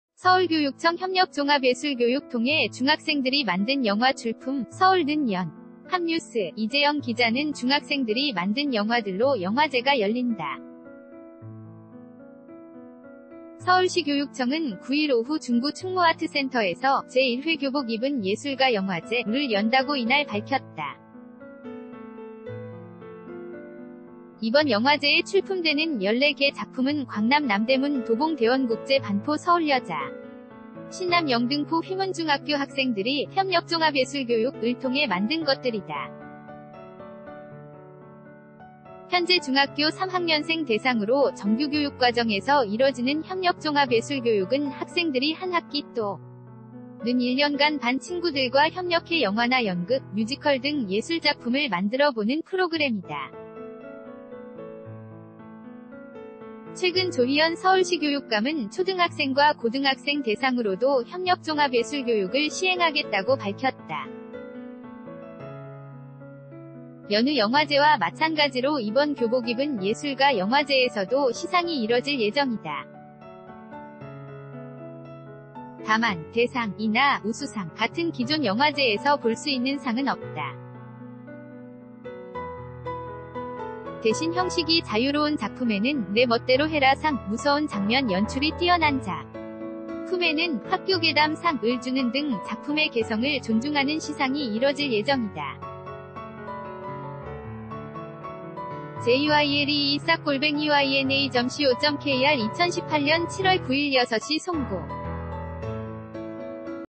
9일 충무아트센터서 '교복 입은 영화감독' 영화제 열려
9일 충무아트센터서 '교복 입은 영화감독' 영화제 열려
Logan Owen